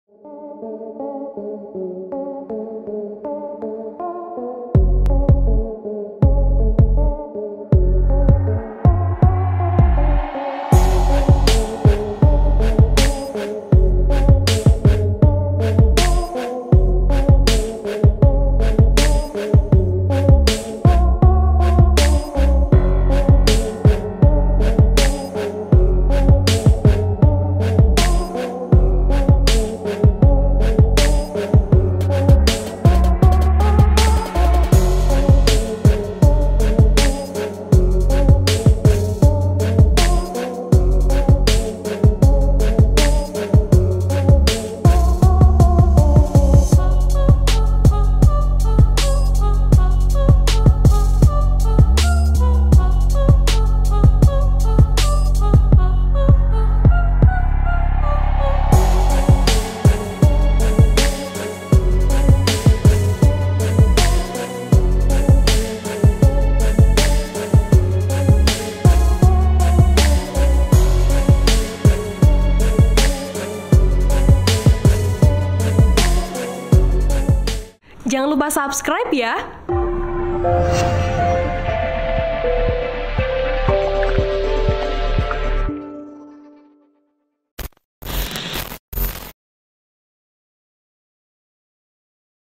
Polres Palu Amankan 5 Terduga Penyalahgunaan Narkoba
TRIBUNVIDEO.COM - Anggota kepolisian kembali mengamankan 5 orang penyalahguna narkoba jenis sabu-sabu di Kota Palu, Kamis (7/11/2019) kemarin.

"Lima orang tersebut diamankan di dua Tkp Kelurahan Tavanjuka, Kecamatan Tatanga, Kota Palu," ungkap Kapolres Palu AKBP H Moch Sholeh melalui Kasat Narkoba IPTU Stefanus Sanam, Jumat (8/11/2019) pagi.

Di TKP pertama polisi mengamankan 3 terduga penyalahgunaan narkoba yakni HO (36), KN (35), dan IM (22).

Baca selengkapnya: